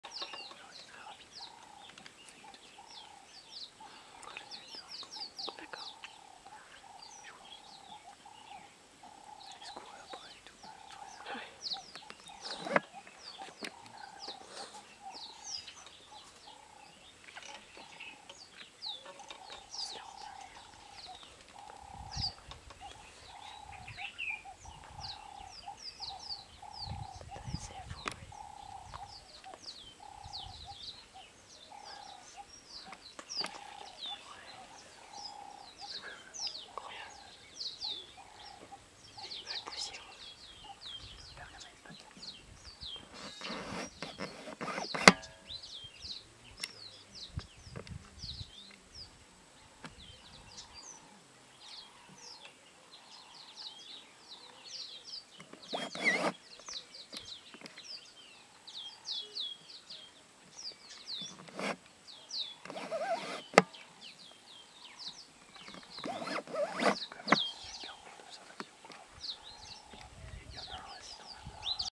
Etosha Halali Waterhole
Une vidéo du trou d'eau de Halali, dans le parc national d'Etosha en Namibie